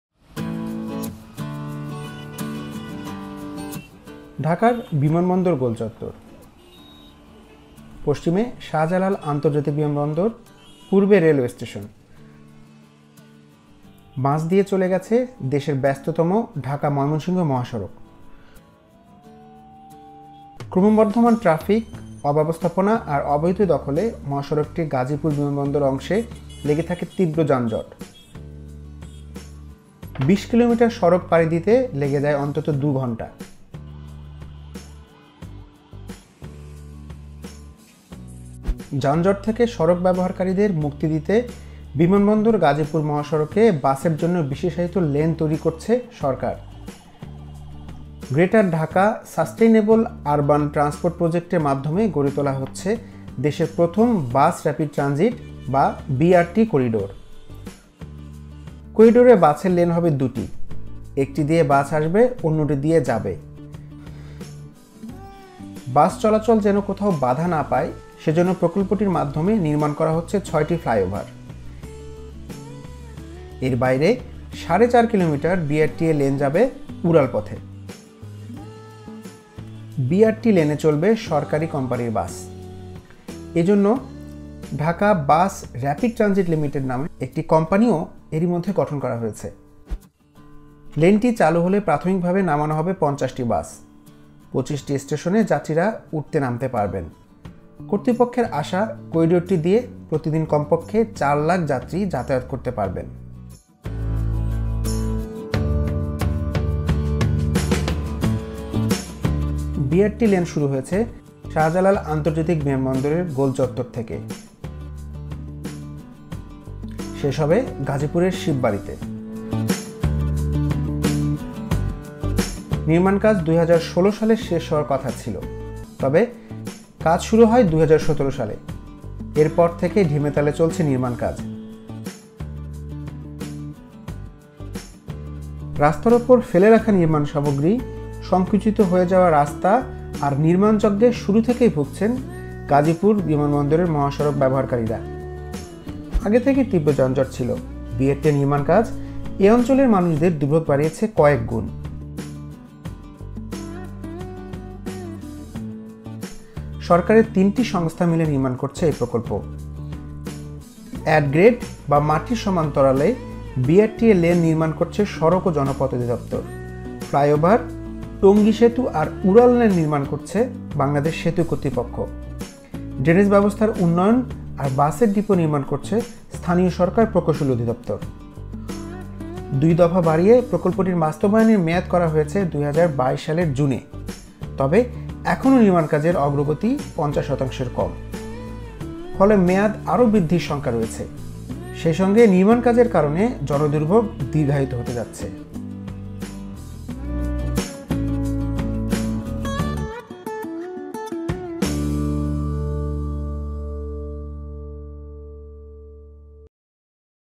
কতদূর এগোলো BRT প্রজেক্ট। সর্বশেষ আপডেট
ঢাকার বিমানবন্দর গোলচত্বরের পশ্চিমে শাহজালাল আন্তর্জাতিক বিমানবন্দর, পূর্বে রেলওয়ে স্টেশন। মাঝ দিয়ে চলে গেছে দেশের ব্যস্ততম ঢাকা-ময়মনসিংহ মহাসড়ক। ক্রমবর্ধমান ট্রাফিক, অ্যবস্থাপনা আর অবৈধ দখলে মহাসড়কটির গাজীপুর-বিমানবন্দর অংশে লেগে থাকে তীব্র যানজট। ২০ কিলোমিটার সড়ক পাড়ি দিতে লেগে যায় অন্তত দুঘণ্টা।
যানজট থেকে সড়ক ব্যবহারকারীদের মুক্তি দিতে বিমানবন্দর-গাজীপুর মহাসড়কে বাসের জন্য বিশেষায়িত লেন তৈরি করছে সরকার। গ্রেটার ঢাকা সাসটেইনেবল আরবান ট্রান্সপোর্ট প্রজেক্টের মাধ্যমে গড়ে তোলা হচ্ছে দেশের প্রথম বাস র্যাপিড ট্রানজিট বা বিআরটি করিডোর। করিডোরে বাসের লেন হবে দুটি। একটি দিয়ে বাস আসবে, অন্যটি দিয়ে যাবে। বাস চলাচল যেন কোথায় বাঁধা না পায়, সেজন্য প্রকল্পটির মাধ্যমে নির্মাণ করা হচ্ছে ছয়টি ফ্লাইওভার। এর বাইরে সাড়ে চার কিলোমিটার বিআরটি লেন যাবে উড়ালপথে।
বিআরটি লেনে চলবে সরকারি কোম্পানির বাস। এজন্য ঢাকা বাস র্যাপিড ট্রানজিট লিমিটেড নামের একটি কোম্পানিও এরইমধ্যে গঠন করা হয়েছে। লেনটি চালু হলে প্রাথমিকভাবে নামানো হবে ৫০টি বাস। ২৫টি স্টেশনে যাত্রীরা উঠতে-নামতে পারবেন। কর্তৃপক্ষের প্রত্যাশা, করিডোরটি দিয়ে প্রতিদিন কমপক্ষে চার লাখ যাত্রী যাতায়াত করতে পারবেন।
আরও দেখুন:
রাজবাড়ীর চারটি গ্রামের যাতায়াতের একমাত্র ভরসা আবেদা বেগম ও তার ভাঙ্গা নৌকা